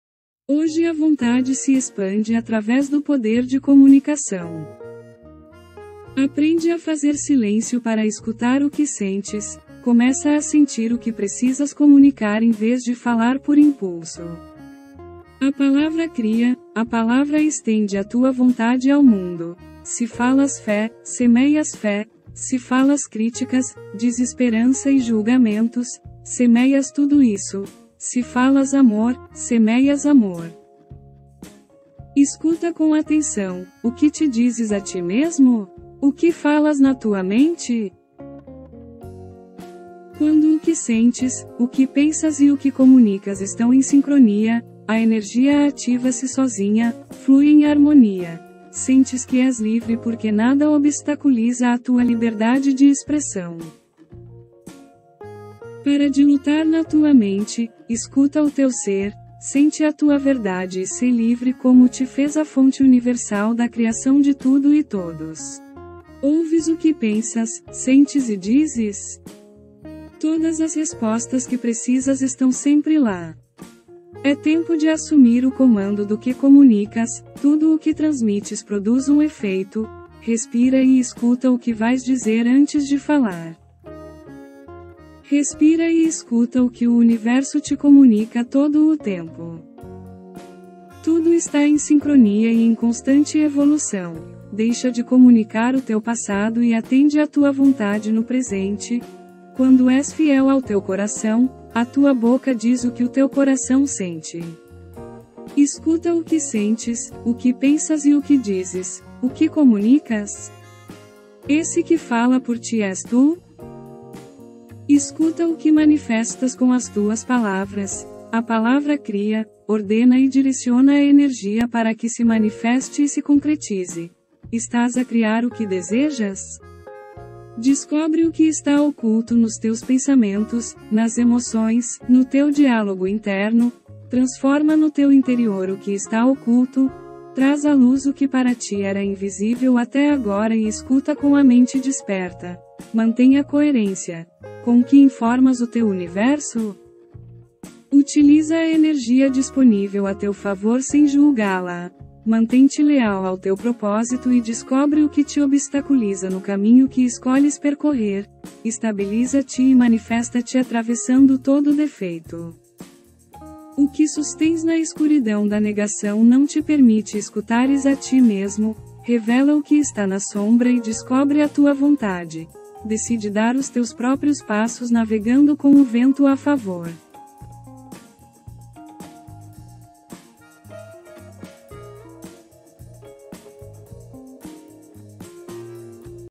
Hoje a vontade se expande através do poder de comunicação. !!!▲[MOTIVACIONAL]
Descubra a sinfonia da comunicação e mergulhe na harmonia da sua vontade interior neste inspirador vídeo do YouTube. Aprenda a fazer silêncio para escutar o que você realmente sente e comece a expressar suas emoções de forma consciente. Neste vídeo, exploraremos como as palavras que escolhemos criarão o mundo ao nosso redor, seja semear fé, amor ou críticas e desesperança. Você vai se surpreender ao perceber como a sincronia entre seus sentimentos, pensamentos e palavras pode ativar uma energia poderosa em sua vida. Junte-se a nós nessa jornada de autodescoberta, onde você aprenderá a se libertar das lutas internas, encontrar sua verdade e comunicá-la com autenticidade. Respire fundo, abra sua mente e deixe o universo comunicar-se com você. Este vídeo o guiará a assumir o controle do que você transmite ao mundo, transformando seus pensamentos ocultos em uma força criadora. Descubra como sua comunicação influencia seu universo e aprenda a superar obstáculos no caminho da sua escolha. Prepare-se para estabilizar-se e manifestar sua verdade, navegando com o vento a seu favor. Não perca essa oportunidade de descobrir o poder transformador da comunicação consciente e a capacidade de criar a realidade que você deseja. Assista agora e deixe-se envolver pela sinfonia da comunicação!